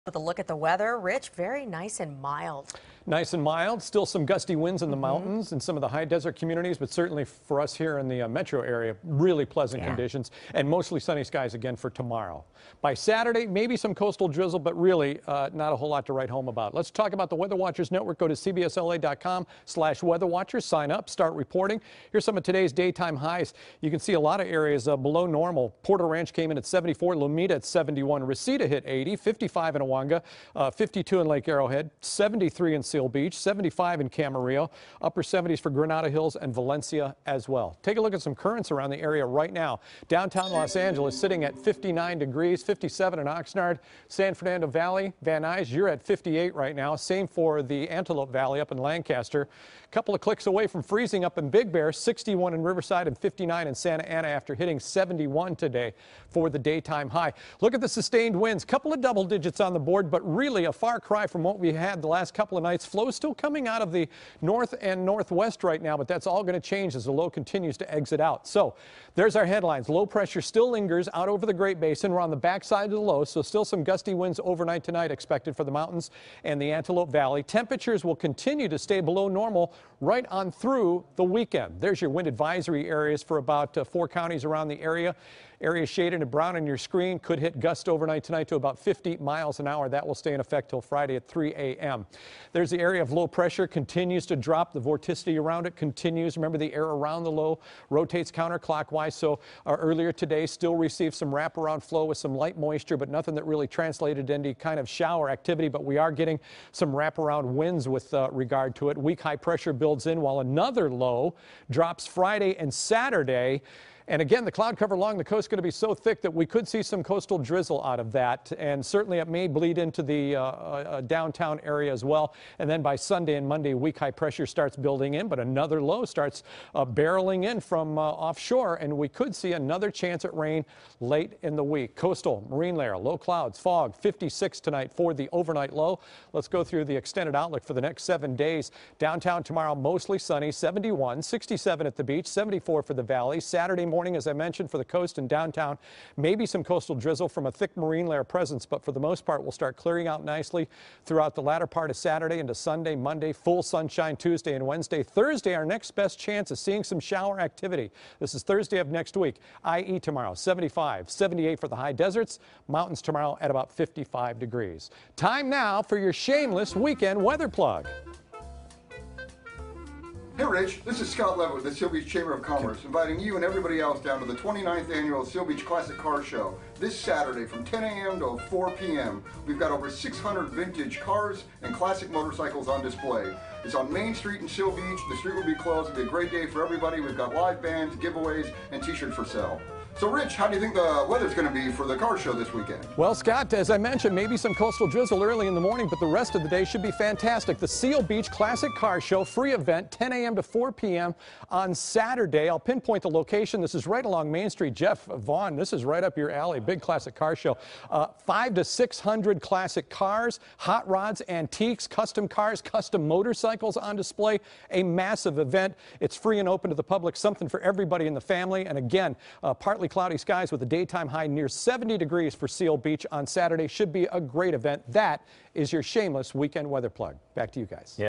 Rich Fields' Weather Forecast (April 28)
Wind advisories have been extended into Friday, with sunny and warm weather expected for the weekend. Rich Fields reports.